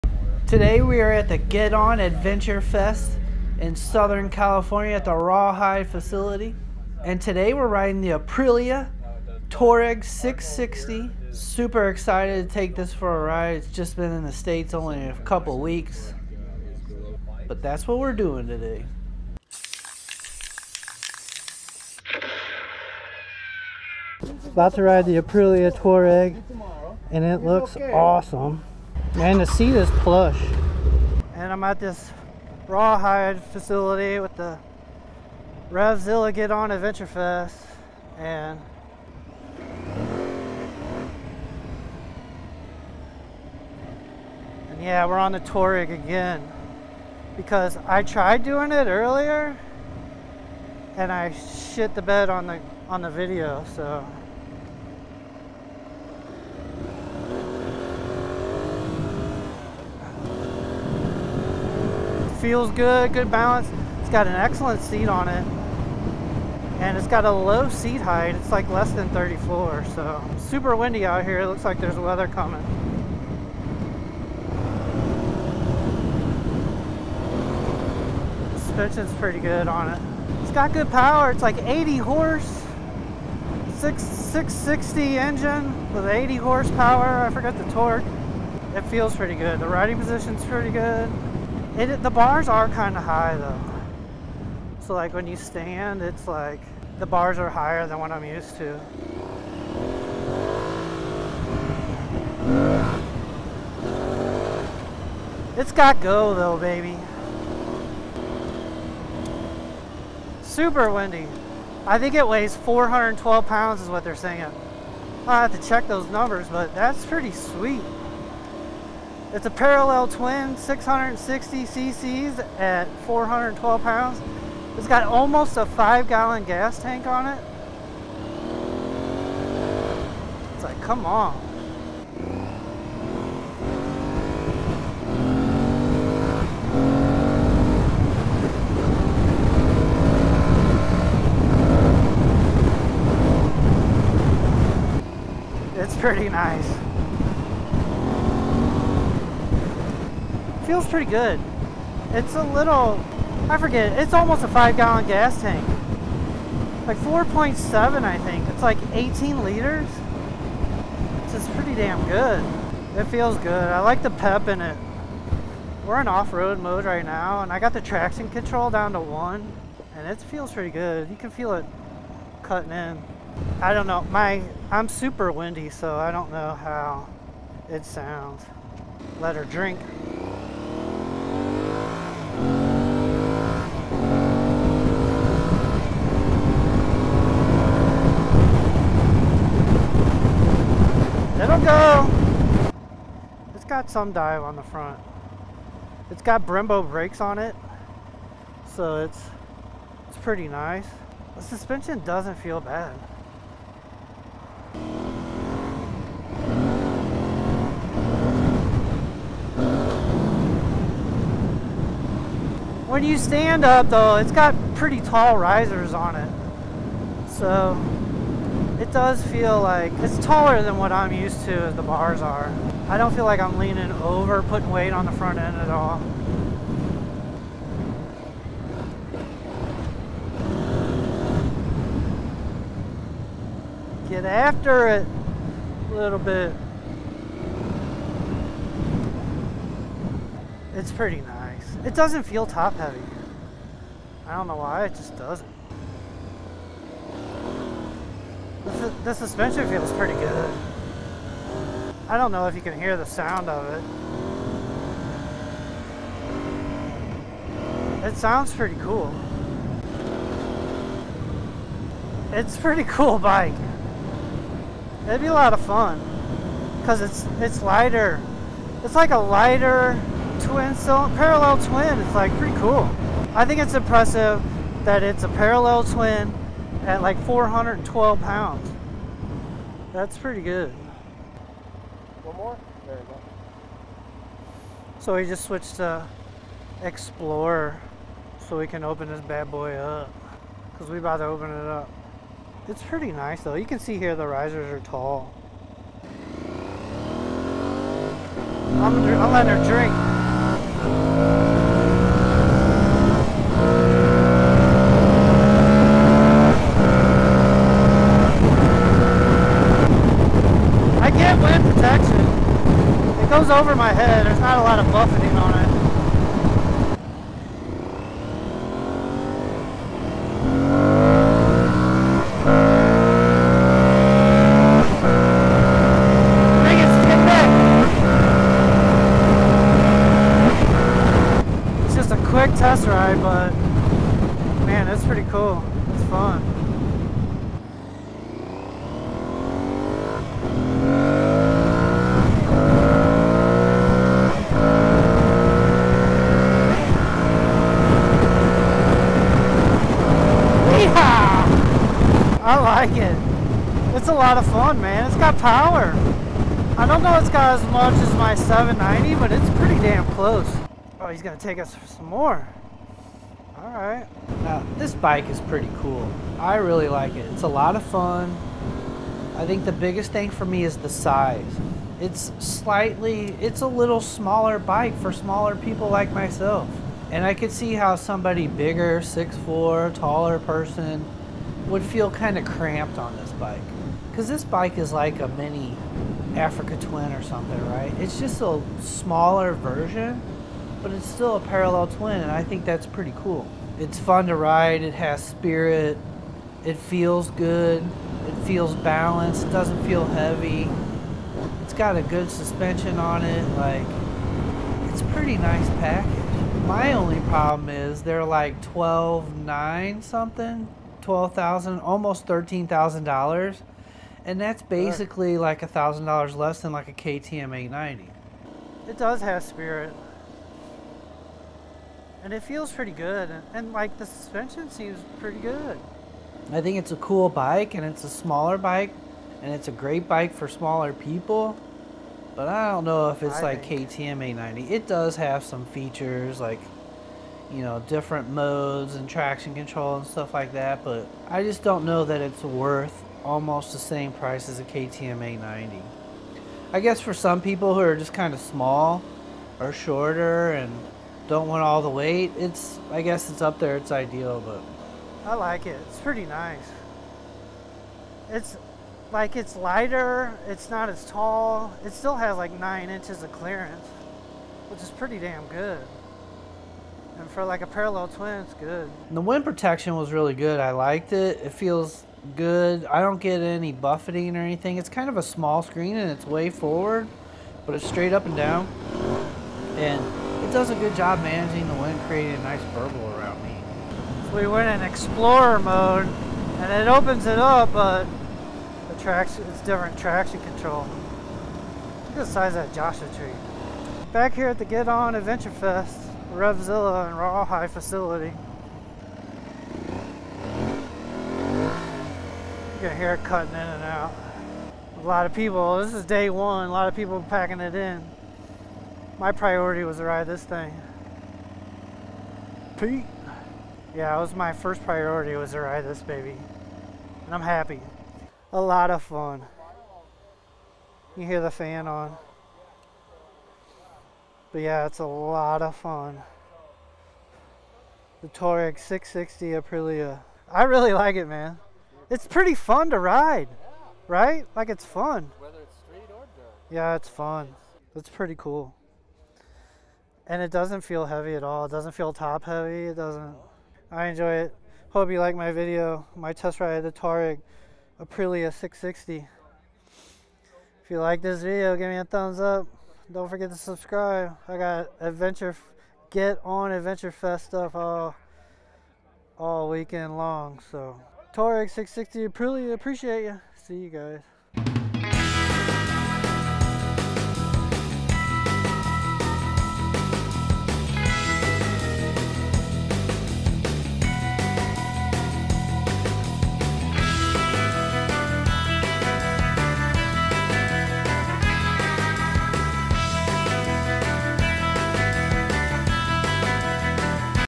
Aprilia TUAREG 660 | First Ride OFF-ROAD and First Impression
aprilia Tuareg 660 First ride off-road and ifirst impression

I got a chance to ride the Aprilia Tuareg 660 when I was at the Get On! Adv Fest presented by @RevZilla

The Tuareg 660 is only been in the US for a couple weeks and I was lucky enough to take it on a test ride on and off-road.
Its a really good bike and performs well in all different terrains.

This bike also sets the standard with the lowest weight of a parallel-twin engine and almost 5 gallons of fuel capacity.

The Tuareg 660 is a pretty cool bike and shows what's possible with lightweight parallel-twin motorcycles.